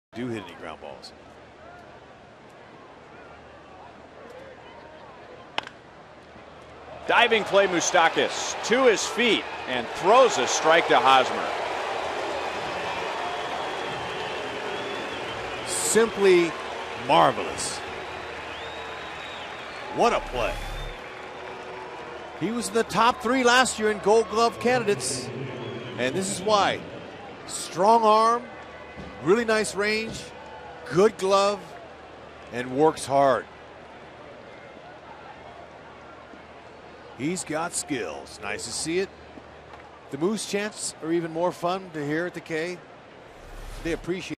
2013/08/22 Moustakas' diving stop
CWS@KC: Moustakas makes an incredible diving stop